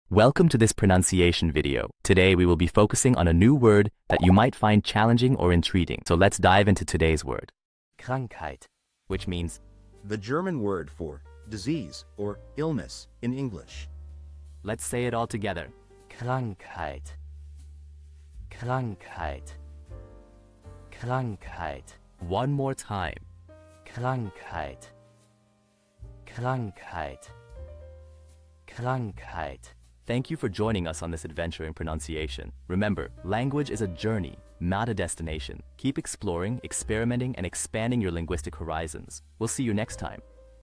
How to pronounce Krankheit
Master the Pronunciation of 'Krankheit which Means illness' - which means : The German Word for "Disease" Or "Illness" in English. 📝 with @PronunciationGerman 🎓🗣️ - [Your Guide to German]

Learn to pronounce 'Krankheit' perfectly in German with our easy and engaging tutorial by @PronunciationGerman! 📖📽️ In this comprehensive video, we provide you with native speaker examples 🗣️👂 and phonetic breakdowns 🎵 to ensure you get the pronunciation just right.

How to say Illness in German